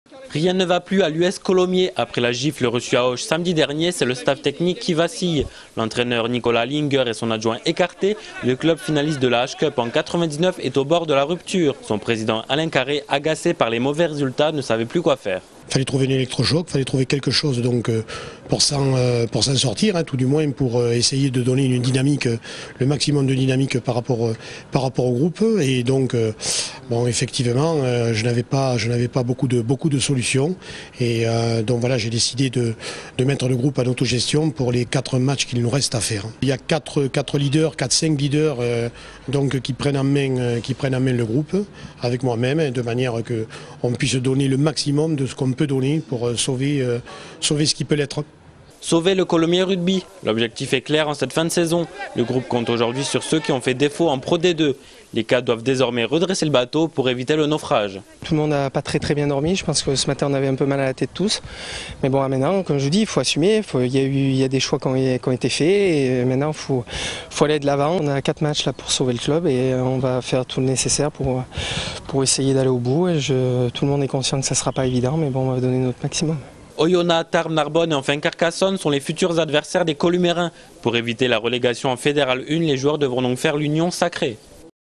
Rugby Pro D2 : Colomiers au bord de la rupture !
Sites webs/blogs, copiez cette vidéo avec le bouton "Intégrer" !
 Nicolas Hallinger et Ludovic Chambriard, le duo d'entraîneurs de Colomiers, 15e de Pro D2, a été écarté par le président Alain Carré. Le groupe est dorénavant "auto géré"
. (DROITS RESERVES . Pour toute exploitation sur TV, web, mobile, aller